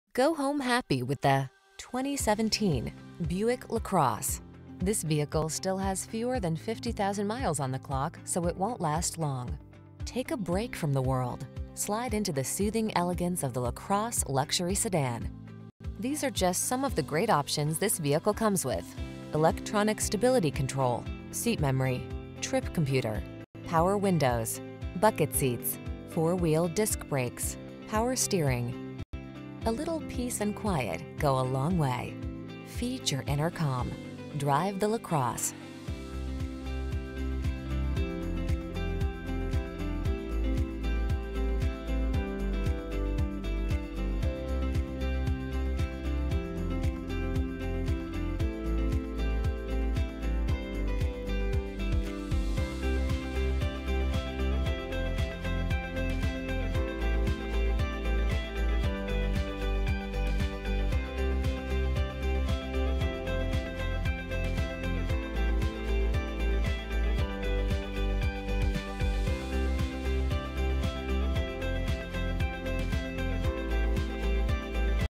2017 Buick LaCrosse Gurnee, Waukegan, Kenosha, Arlington Heights, Libertyville, IL 13611
Black Onyx [LISTING TYPE] 2017 Buick LaCrosse available in Gurnee, Illinois at Anthony Buick. Servicing the Waukegan, Kenosha, Arlington Heights, Libertyville, IL area.